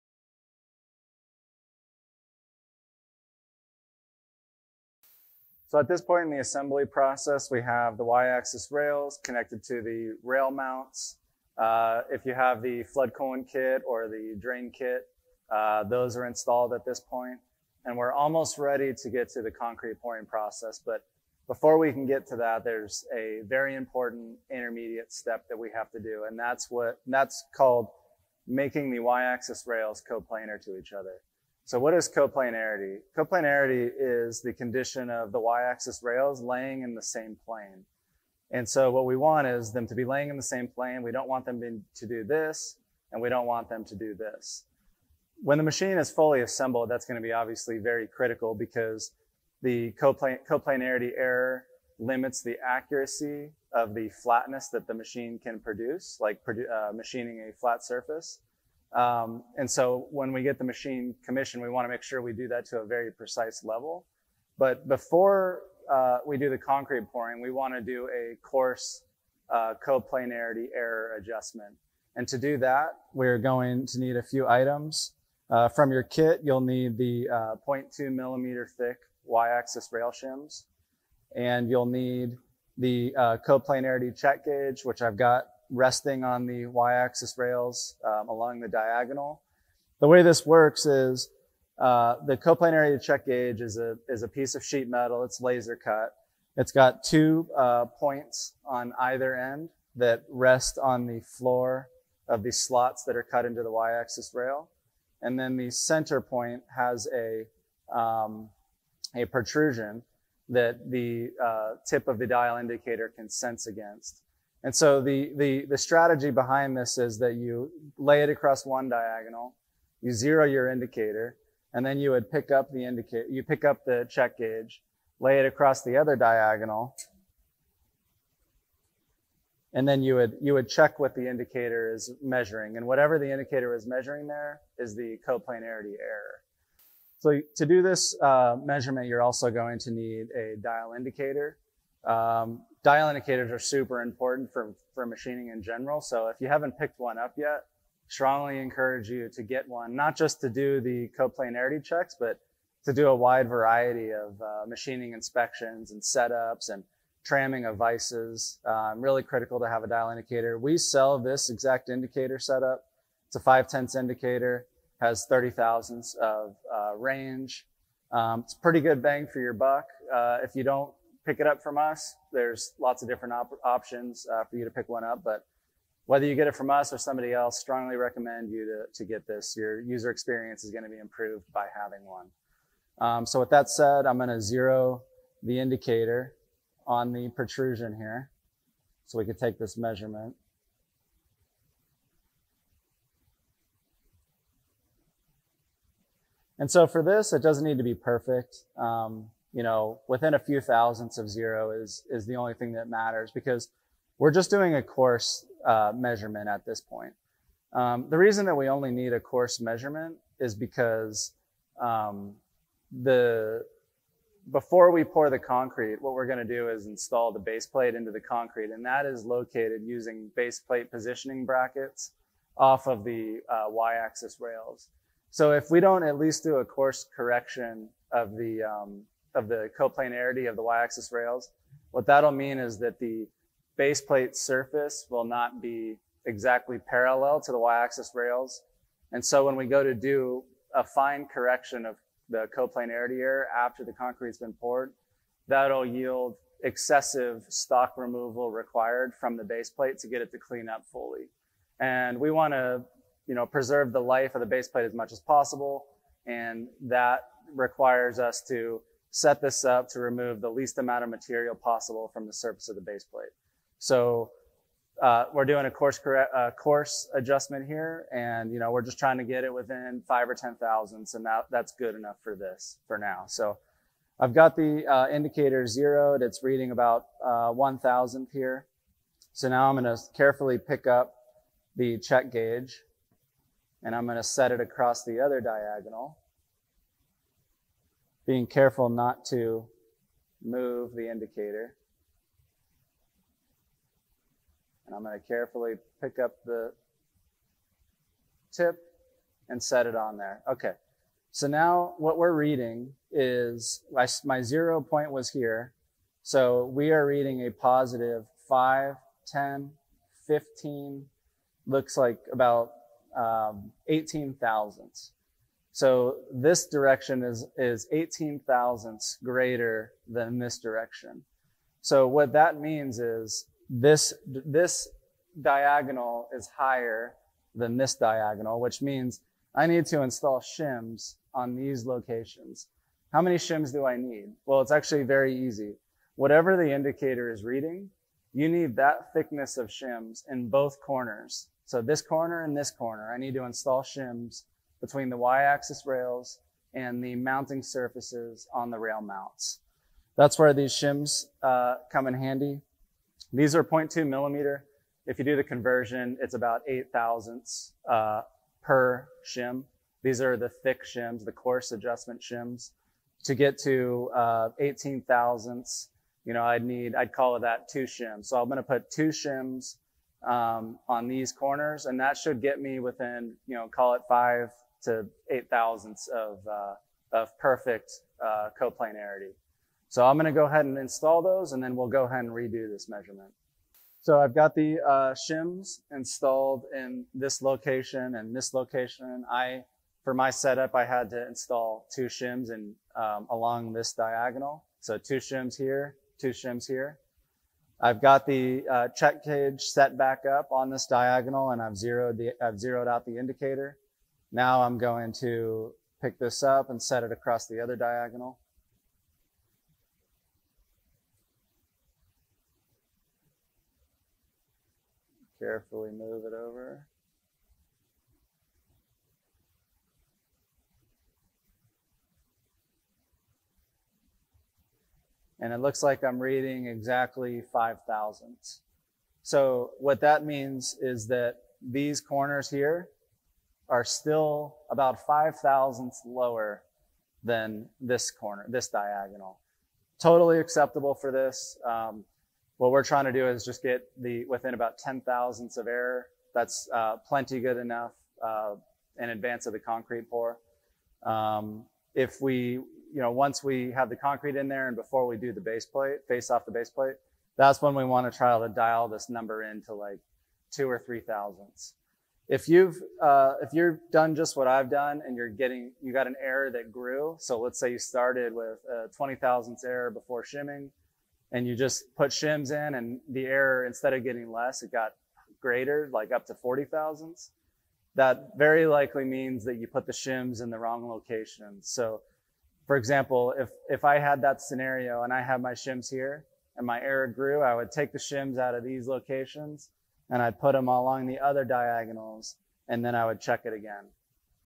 MR-1 CNC Mill Assembly Tutorial: Y-axis Rail Coarse Adjustment
In this video we show the steps needed to make coarse adjustments to the Y-axis rails on MR-1 before concrete is poured into the base. For more information regarding the complete assembly steps for MR-1, please refer to our online Assembly Manual.

To learn more about the Concrete Base design on MR-1, please check out the article on our